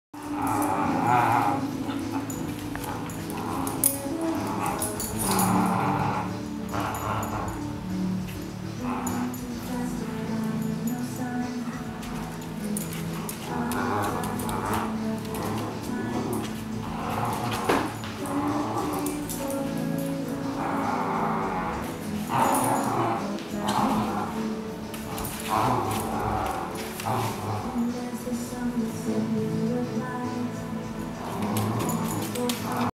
Dogbook Movie: Tug-of-War
Dixon playing tug-of-war with Lola